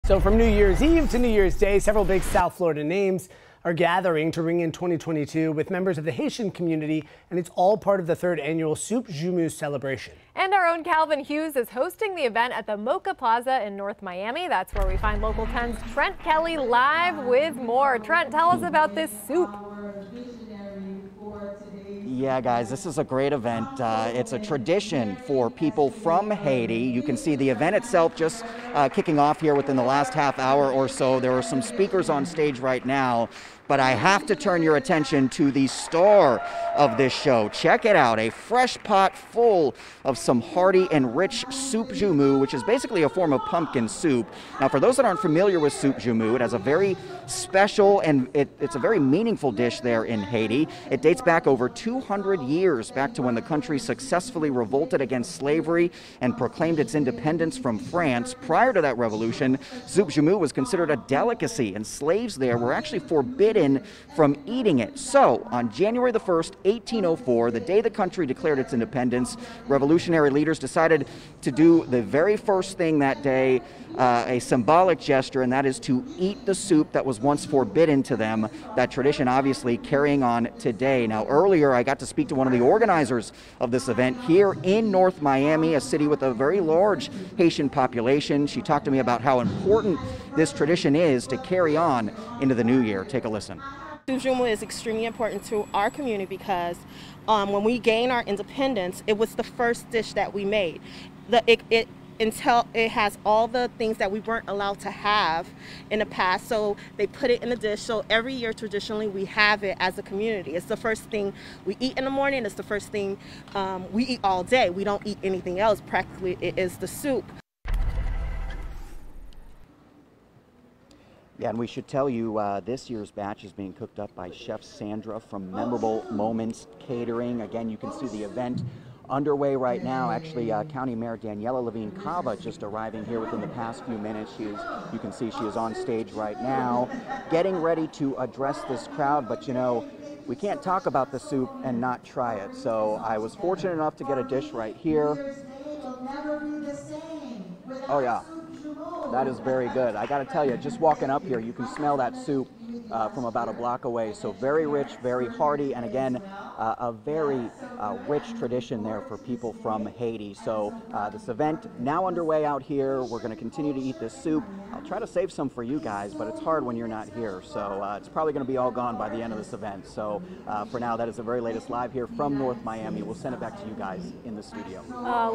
Haitian-Americans celebrate Haiti’s Independence Day in North Miami
Haitian-Americans gathered in North Miami Saturday to celebrate Haiti’s Independence Day at the third annual Soup Joumou celebration.